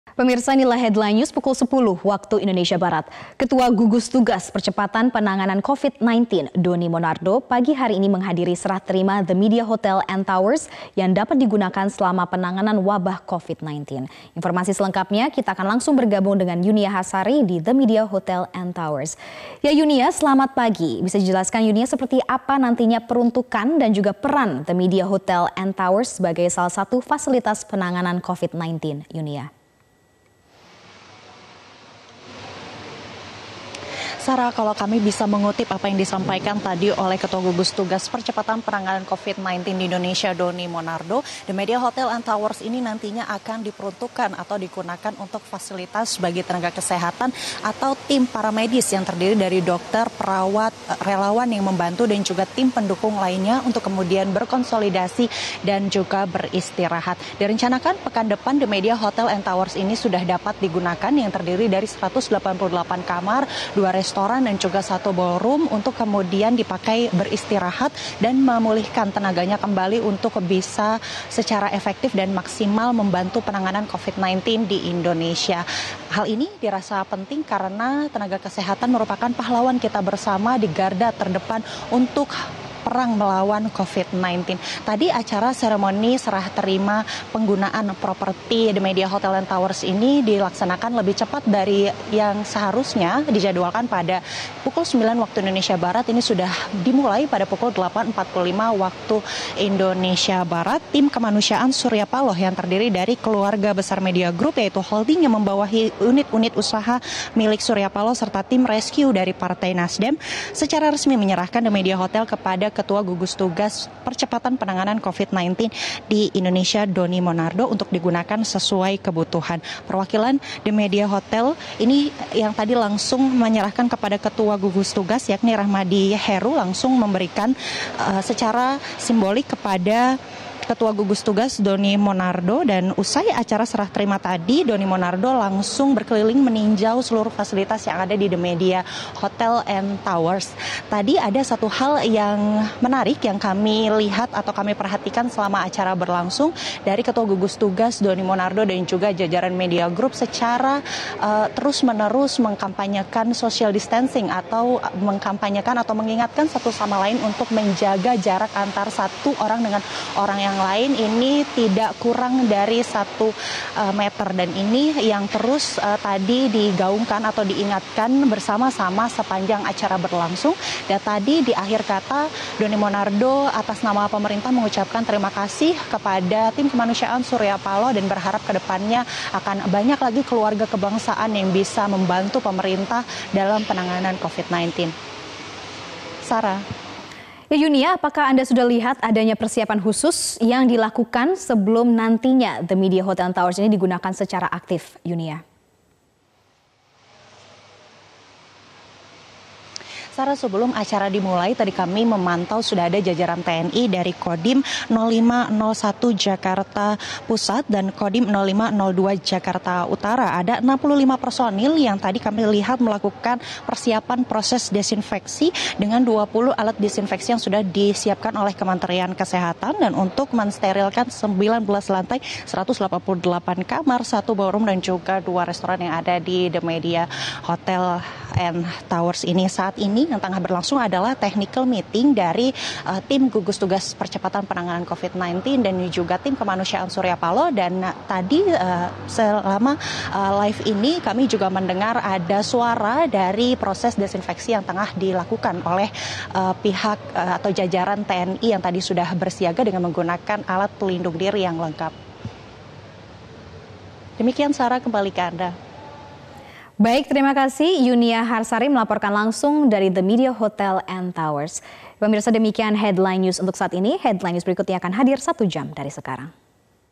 Serah Terima Penggunaan The Media Hotel untuk Tenaga Medis Covid-19
Ketua Gugus Tugas Percepatan Penanganan covid-19 Doni Monardo menghadiri serah terima The Media Hotel and Towers yang dapat digunakan sebagai tempat beristirahat tenaga medis selama penanganan covid-19. Rencananya The Media Hotel and Towers ini dapat digunakan pekan depan.